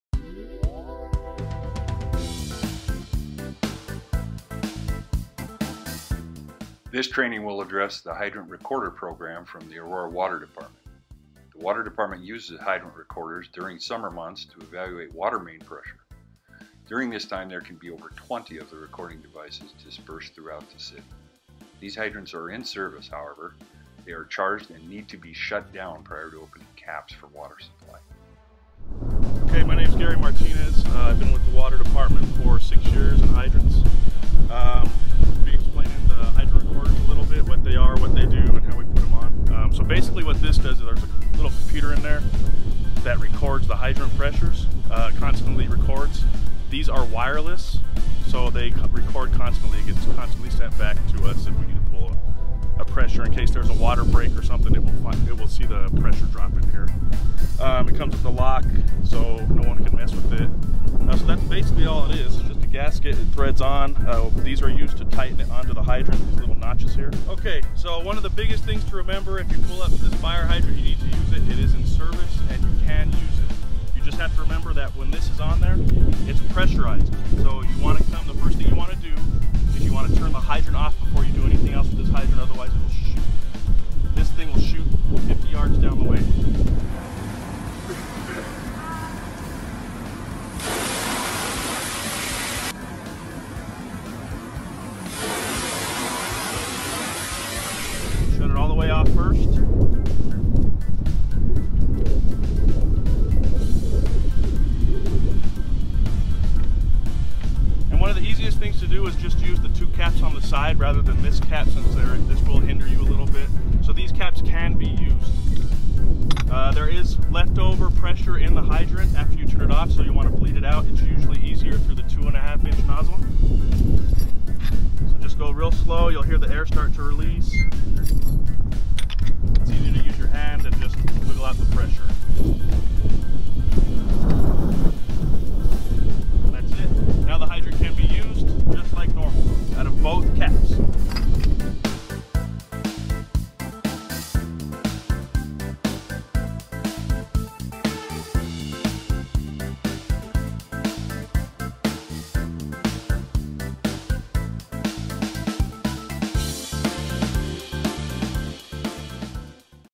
Hydrant Recording Device Aurora Fire
How to establish a water supply in the presence of a Hydrant Recording Device.